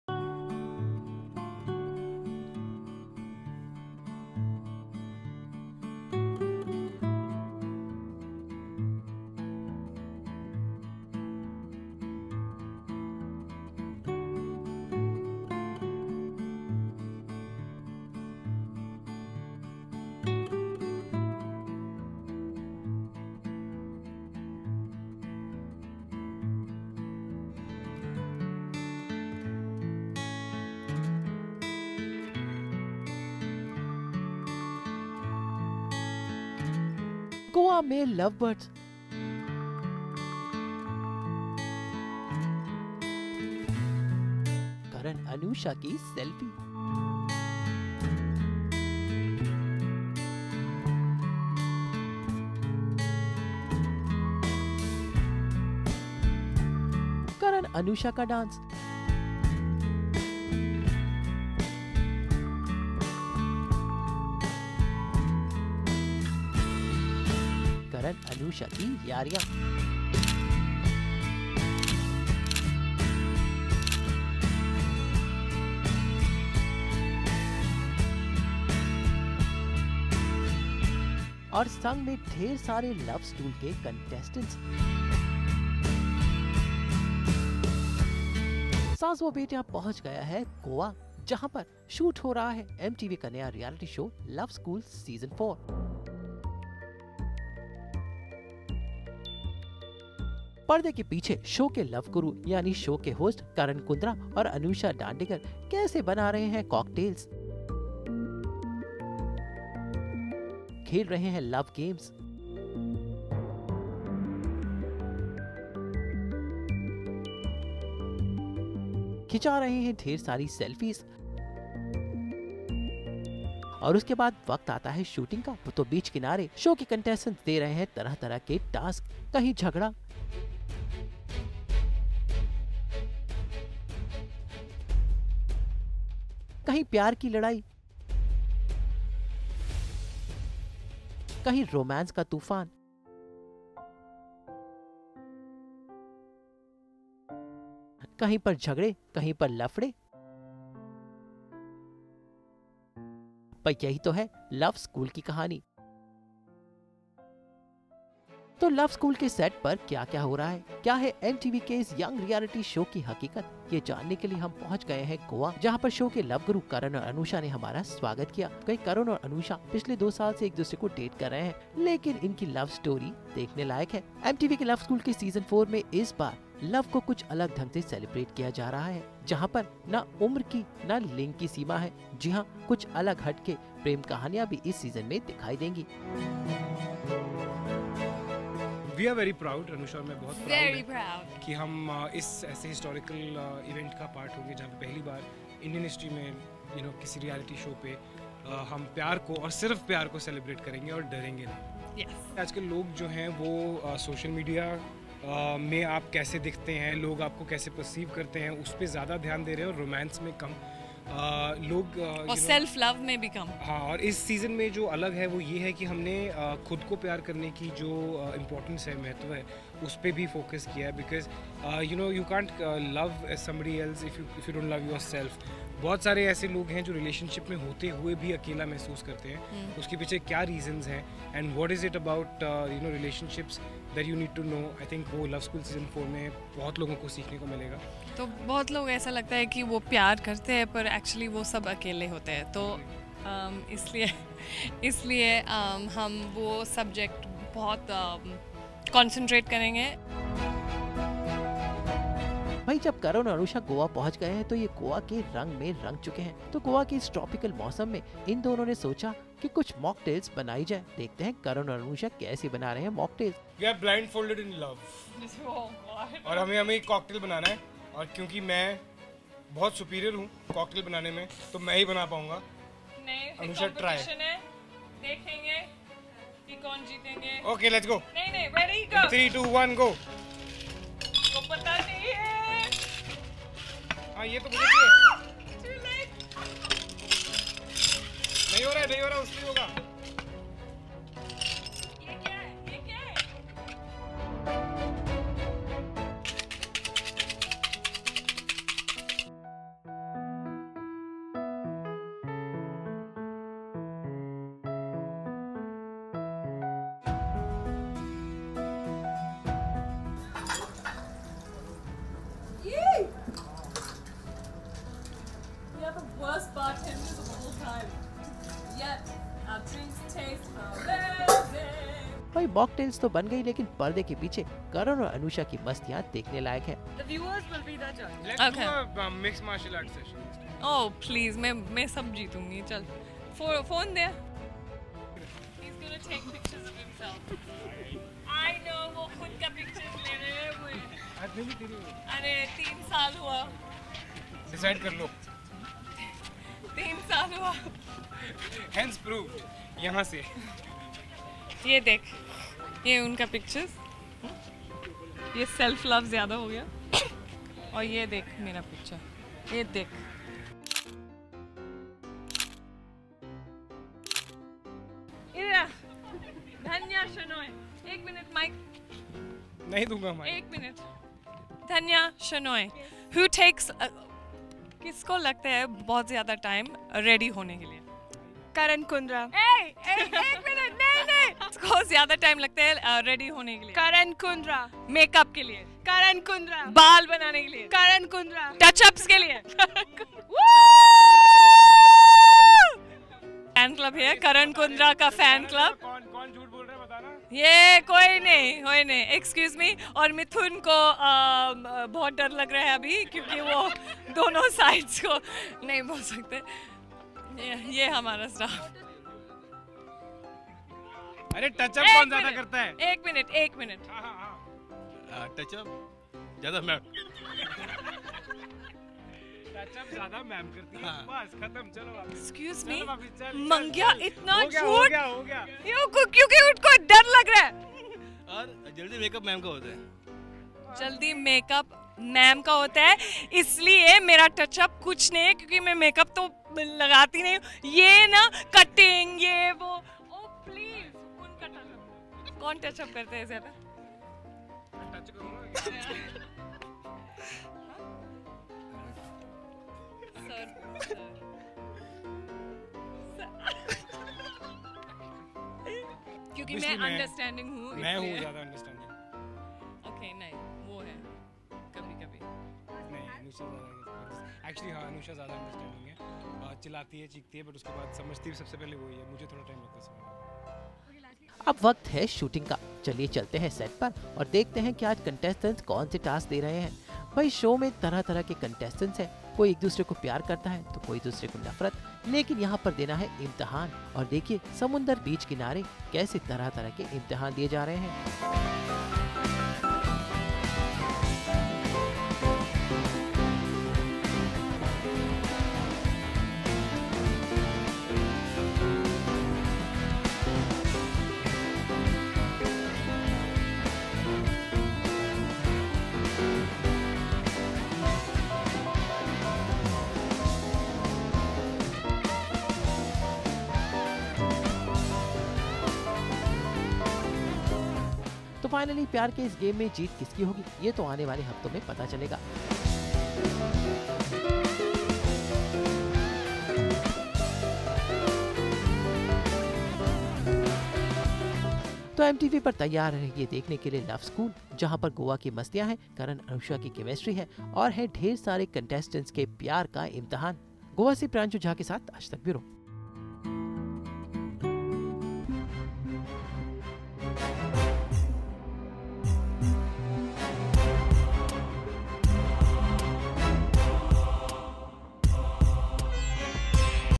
Goa Dayout With Love School's Host Karan Kundra & Anusha Dandekar!
Checkout a fun filled dayout with Love School's host Karan Kundra & Anushka Dandekar in this video..